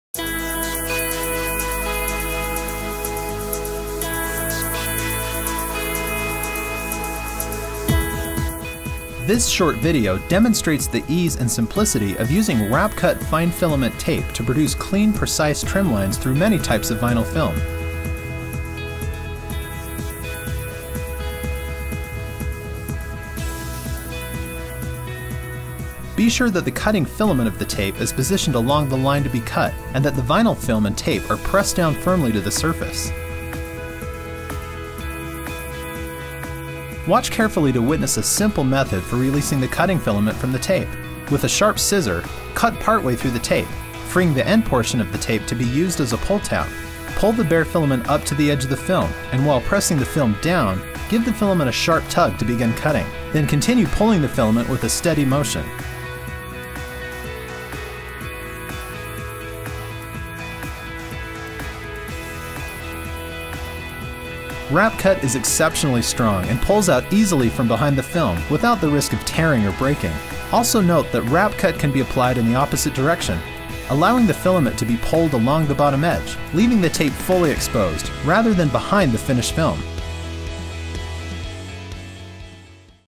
WrapCut® 101 [ Straight Lines and Basic Instruction ]
In our original demonstration video, you can view how easy it is to use WrapCut™ Fine Filament Edge Cutting Tape for cutting vinyl vehicle wraps.